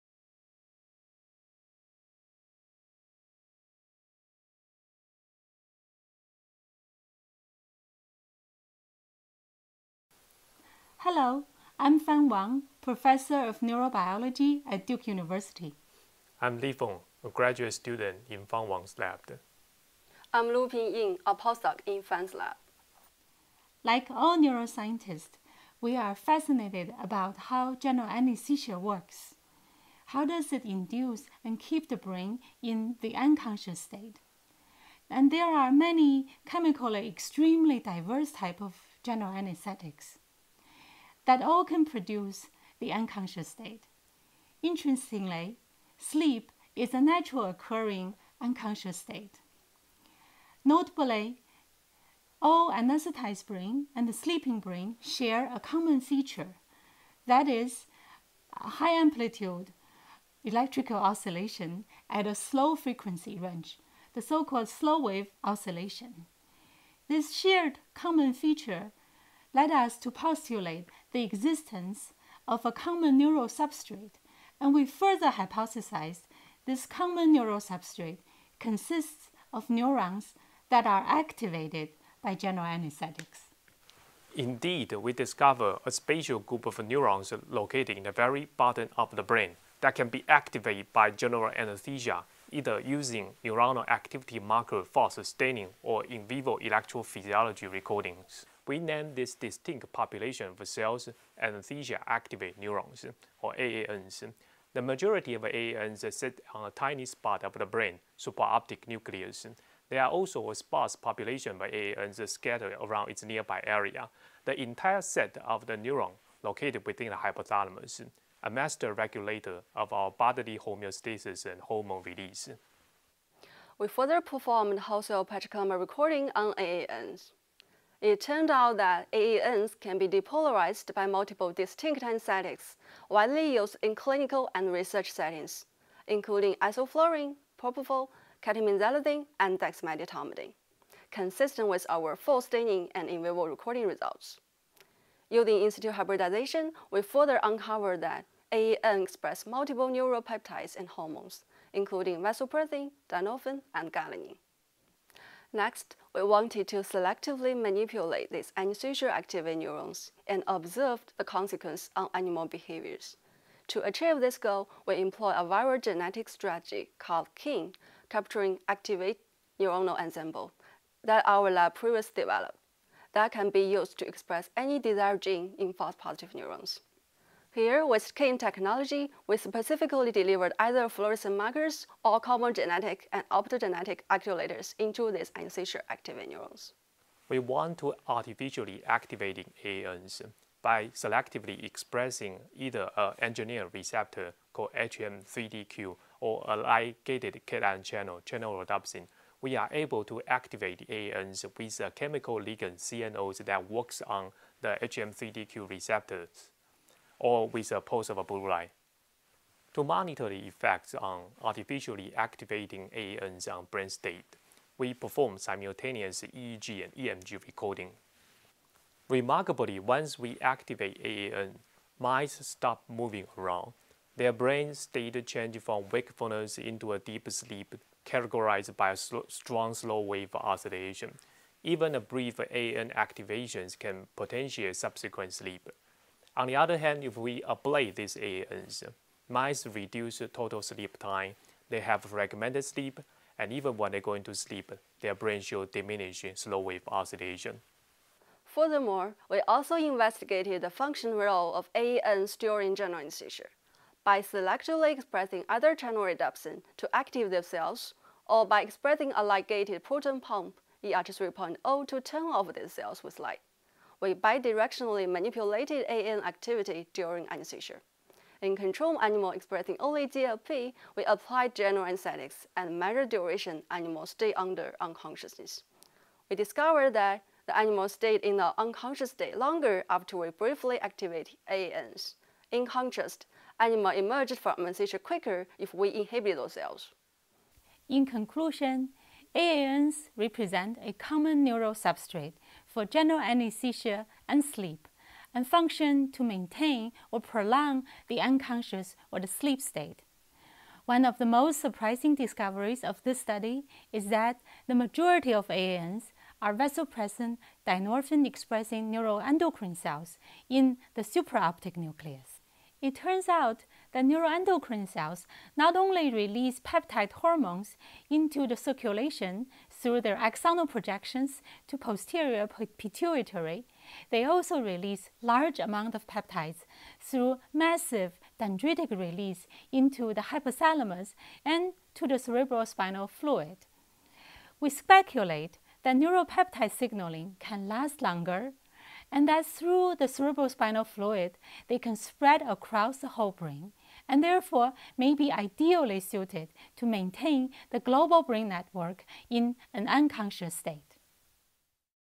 An Active Circuit for Diverse Anesthetics and Sleep
This study discovered a common neuroendocrine substrate activated by diverse general anesthetics and is also critically required for natural sleep. These anesthesia-activated neurons can strongly potentiate slow-wave sleep and extend the duration of anesthesia.

Jiang-Xie, L.F., Yin, L., Zhao, S., Prevosto, V., Han, B.X., Dzirasa, K., and Wang, F. (2019). A Common Neuroendocrine Substrate for Diverse General Anesthetics and Sleep. Neuron. 102.